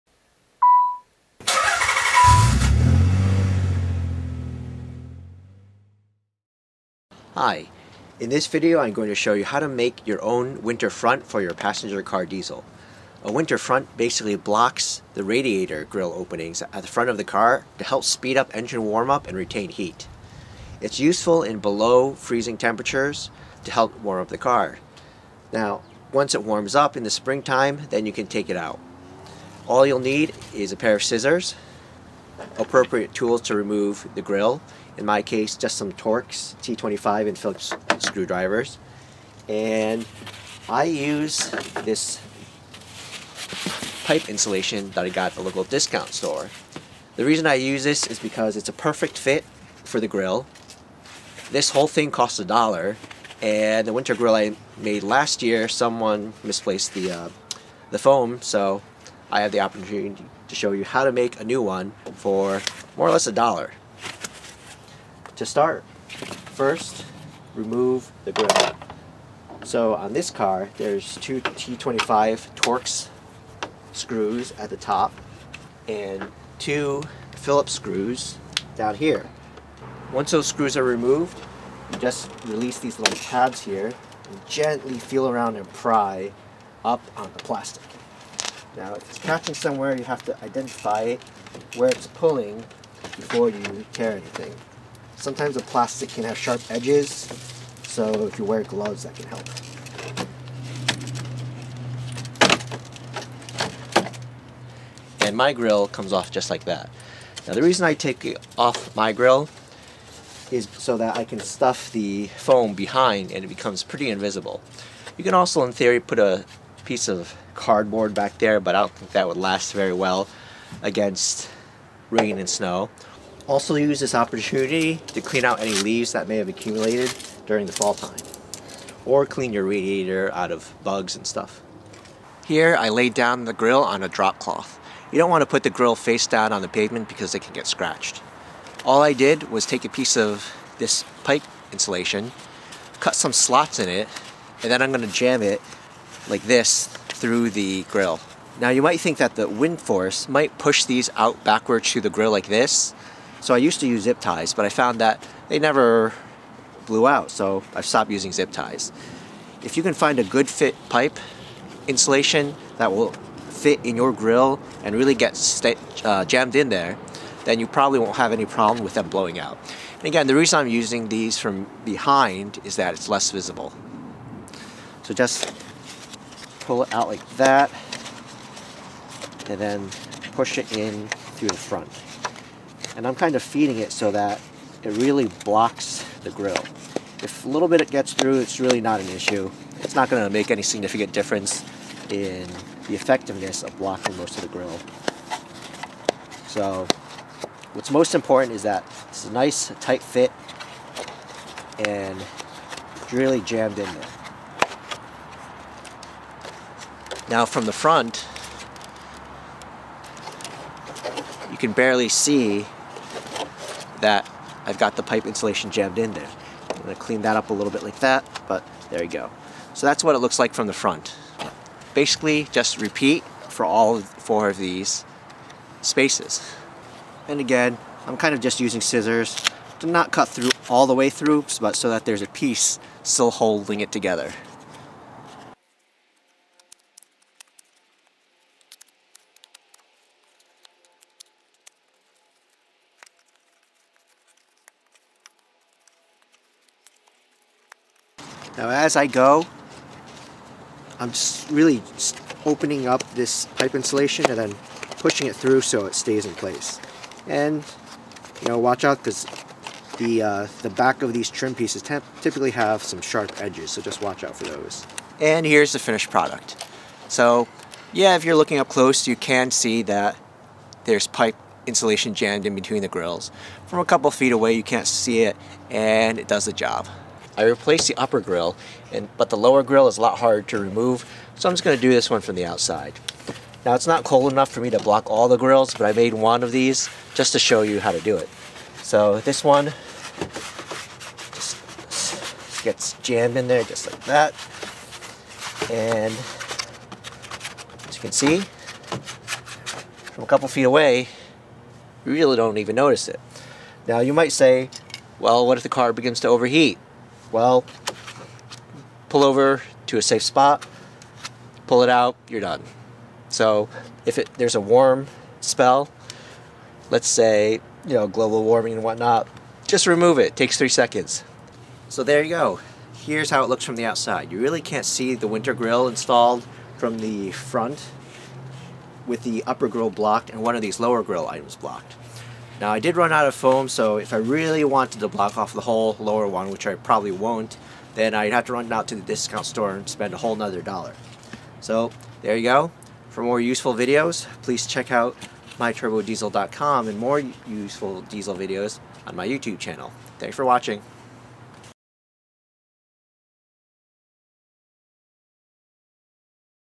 DIY winter front grille foam barrier installation on a diesel
This video shows how to install a DIY winter front grille.  I used foam pipe insulation to block the grille.  You can also just do the lower but I wanted to keep it hidden.